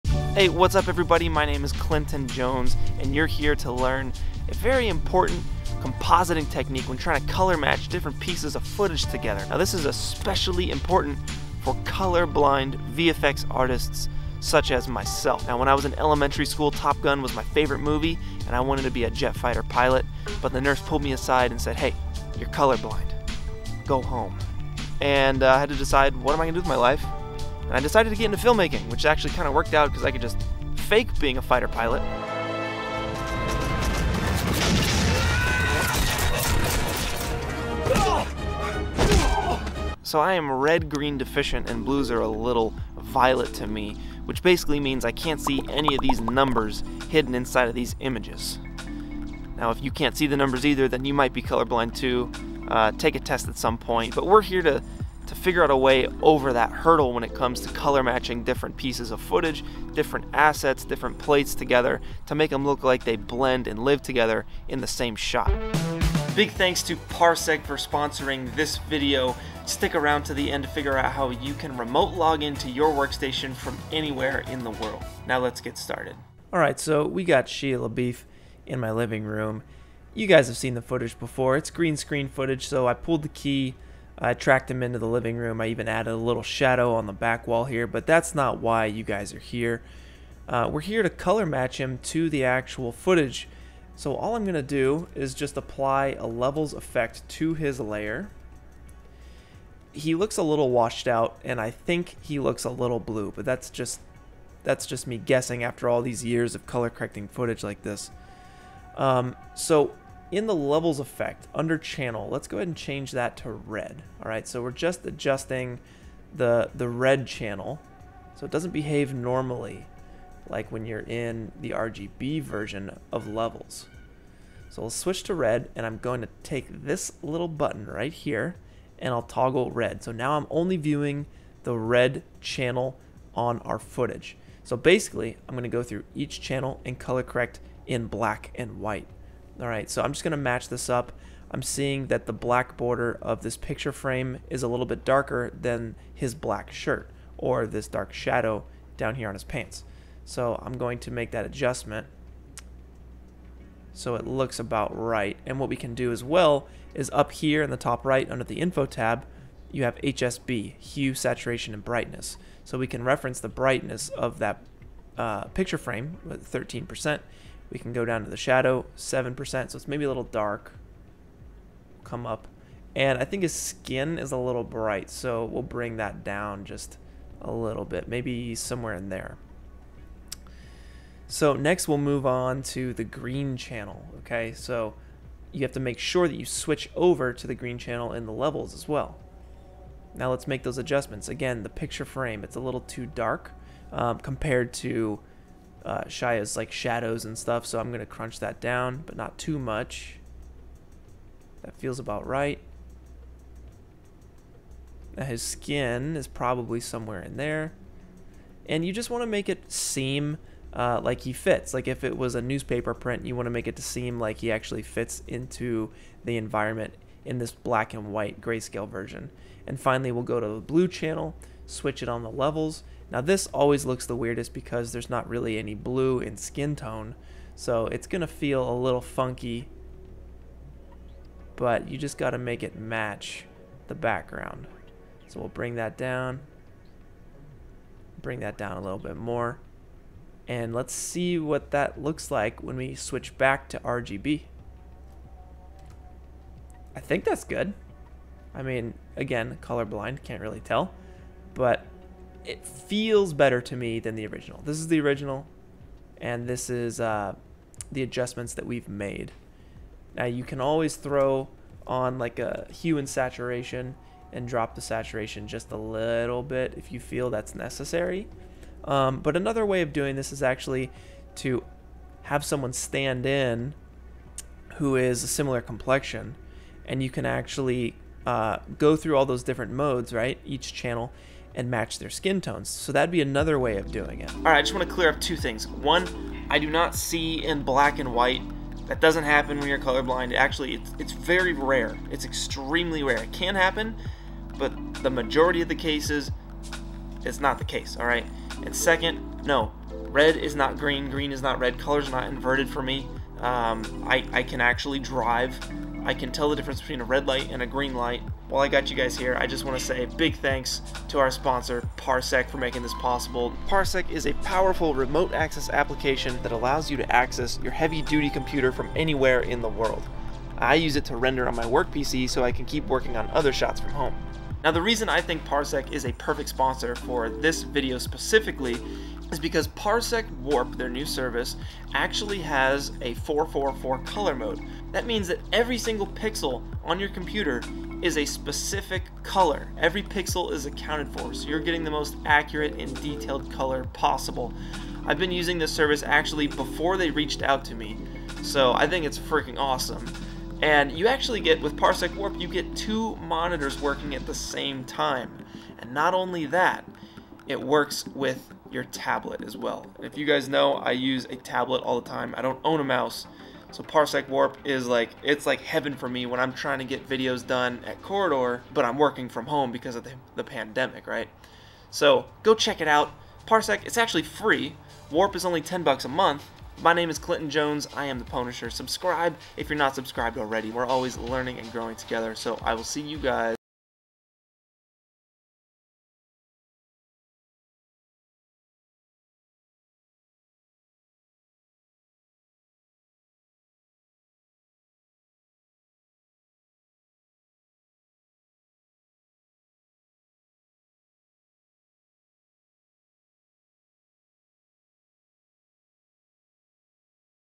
Advanced Color Matching Trick in After Effects | Color Blind VFX
This technique is for my colorblind vfx artists out there who have as much trouble compositing their footage as I do. But you don't have to be color-blind to use this tip, so let's dive into AfterEffects together.

CHAPTERS:
0:00 Intro
0:15 How I Discovered I was Color Blind
0:40 How My Color Blindness Works
1:15 Color Blind Compositing in After Effects
4:44 Color Blind Myths
5:09 PARSEC Warp: How it Helps me Work